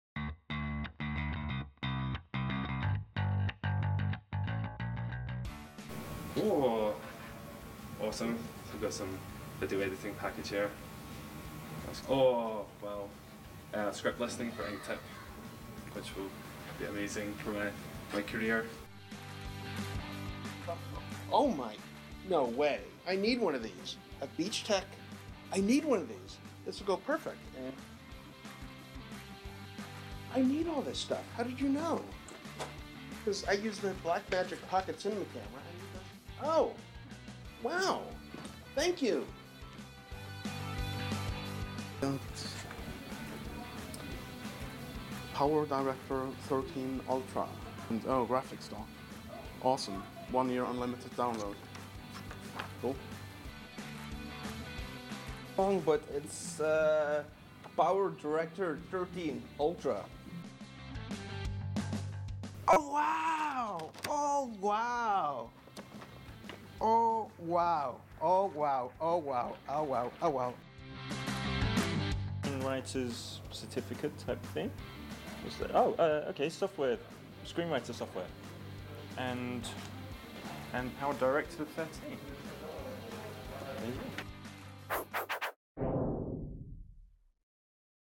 ÉCU 2015 - WINNER'S REACTION
Winner's reaction to receiving their prizes during ÉCU 2015